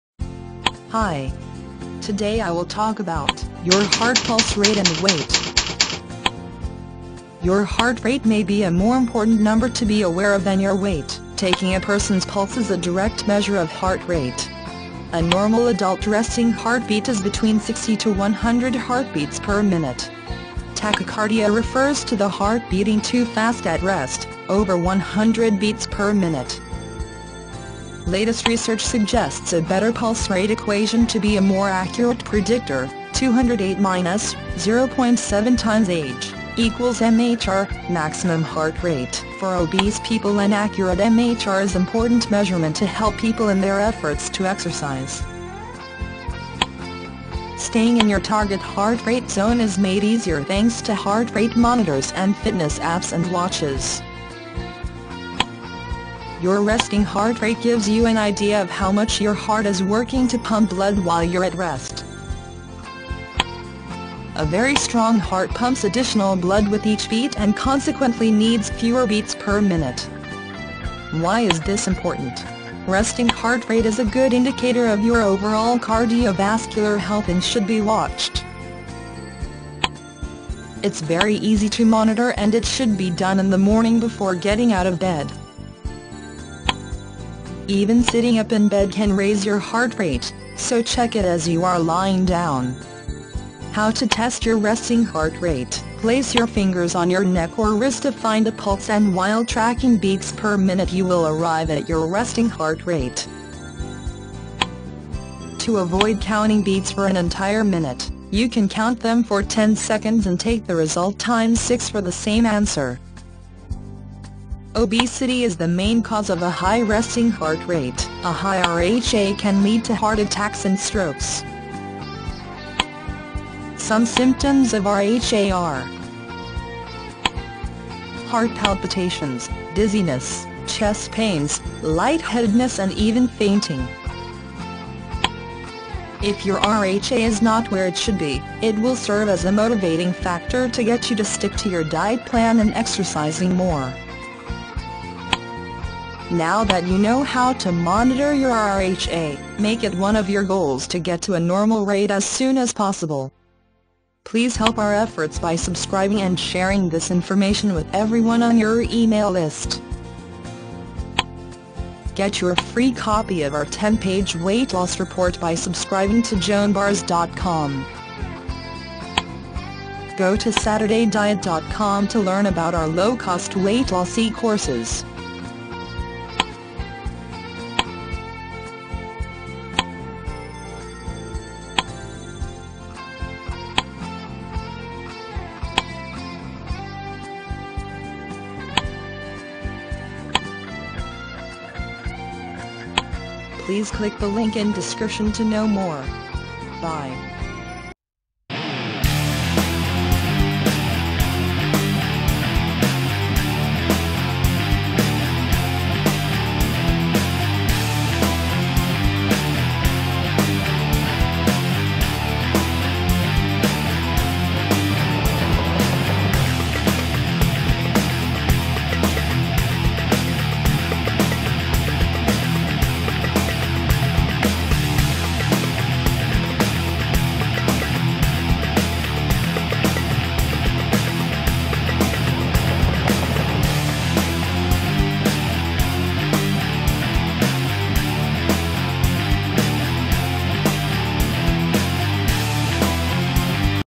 Your Heart Pulse Rate And Weight
Your heart rate may be a more important number to be aware than your weight.

Taking a person's pulse is a direct measure of heart rate. A normal adult resting heart beat is between 60-100 heartbeats per minute. Some experienced athletes may see their resting heart rate fall below 60 beats per minute. Tachycardia refers to the heart beating too fast at rest - over 100 beats per minute.

Latest research suggests a pulse rate equation to be a more accurate predictor: 208-(0.7 x age)=MHR (Maximum Heart Rate) For obese people an accurate MHR is important measurement to help people in their efforts to exercise.

Staying in your “THRZ” (target heart rate zone) is made easier thanks to heart rate monitors and fitness app’s and watches.

Your resting heart rate (or RHR) gives you an idea of how much your heart is working to pump blood while you’re at rest.  A very strong heart pumps additional blood with each beat and consequently needs fewer beats per minute. Why is this important? Resting Heart Rate is a good indicator of your overall cardiovascular health and should be watched.  It’s very easy to monitor and it should be done in the morning before getting out of bed. Even sitting up in bed can raise your heart rate, so check it as you are lying down.
To test your RHR place your fingers on your neck or wrist to find a pulse and while tracking beats per minute you will arrive at your resting heart rate. To avoid counting beats for an entire minute, you can count them for ten seconds and take the result times 6 for the same answer.

Obesity is the main cause of a high resting heart rate.  A high RHA can lead to heart attacks and strokes.

Some symptoms of RHA are:
Heart Palpitations, dizziness, chest pains, lightheadedness and even fainting.

If your RHA is not where it should be, it will serve as a motivating factor to get you to stick to your diet plan and exercising more.